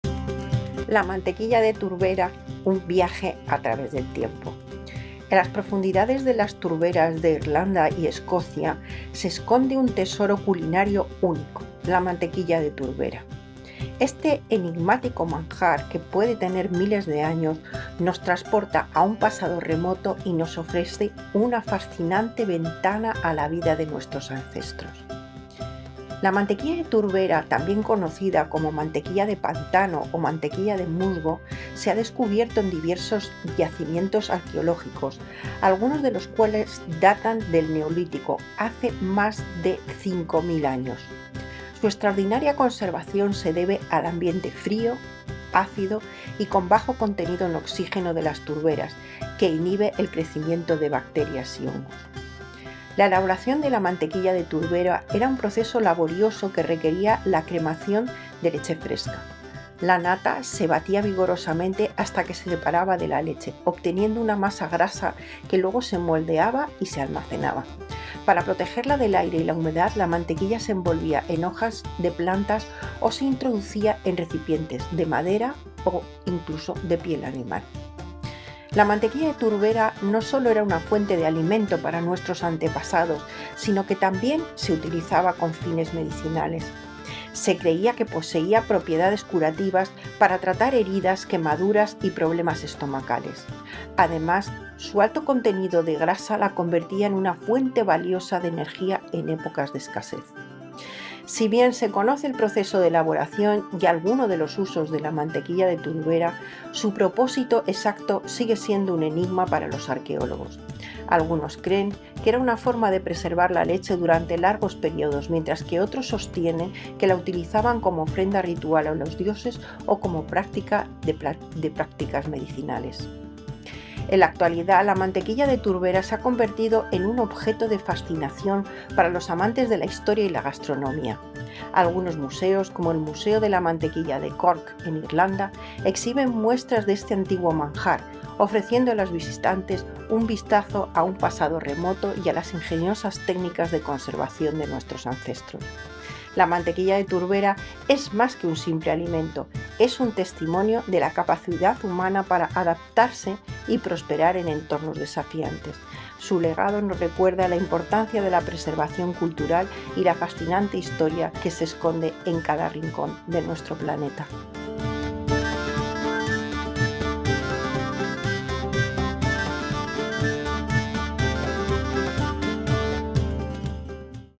La mantequilla de turbera: Un viaje inesperado a través del tiempo
La mantequilla de turbera: Un viaje a través del tiempo

En las profundidades de las turberas de Irlanda y Escocia, se esconde un tesoro culinario único: la mantequilla de turbera. Este enigmático manjar, que puede tener miles de años, nos transporta a un pasado remoto y nos ofrece una fascinante ventana a la vida de nuestros ancestros.

Un legado prehistórico

La mantequilla de turbera, también conocida como "mantequilla de pantano" o "mantequilla de musgo", se ha descubierto en diversos yacimientos arqueológicos, algunos de los cuales datan del Neolítico, hace más de 5.000 años. Su extraordinaria conservación se debe al ambiente frío, ácido y con bajo contenido de oxígeno de las turberas, que inhibe el crecimiento de bacterias y hongos.

Un proceso de elaboración milenario

La elaboración de la mantequilla de turbera era un proceso laborioso que requería la cremación de leche fresca. La nata se batía vigorosamente hasta que se separaba de la leche, obteniendo una masa grasa que luego se moldeaba y almacenaba. Para protegerla del aire y la humedad, la mantequilla se envolvía en hojas de plantas o se introducía en recipientes de madera o piel de animal.

Un tesoro culinario y medicinal

La mantequilla de turbera no solo era una fuente de alimento para nuestros antepasados, sino que también se utilizaba con fines medicinales. Se creía que poseía propiedades curativas para tratar heridas, quemaduras y problemas estomacales. Además, su alto contenido de grasa la convertía en una fuente valiosa de energía en épocas de escasez.

Un enigma arqueológico

Si bien se conoce el proceso de elaboración y algunos de los usos de la mantequilla de turbera, su propósito exacto sigue siendo un enigma para los arqueólogos. Algunos creen que era una forma de preservar la leche durante largos períodos, mientras que otros sostienen que se utilizaba como ofrenda ritual a los dioses o como parte de prácticas medicinales.

Un legado que perdura

En la actualidad, la mantequilla de turbera se ha convertido en un objeto de fascinación para los amantes de la historia y la gastronomía. Algunos museos, como el Museo de la Mantequilla de Cork en Irlanda, exhiben muestras de este antiguo manjar, ofreciendo a los visitantes un vistazo a un pasado remoto y a las ingeniosas técnicas de conservación de nuestros ancestros.

La mantequilla de turbera es más que un simple alimento; es un testimonio de la capacidad humana para adaptarse y prosperar en entornos desafiantes. Su legado nos recuerda la importancia de la preservación cultural y la fascinante historia que se esconde en cada rincón de nuestro planeta.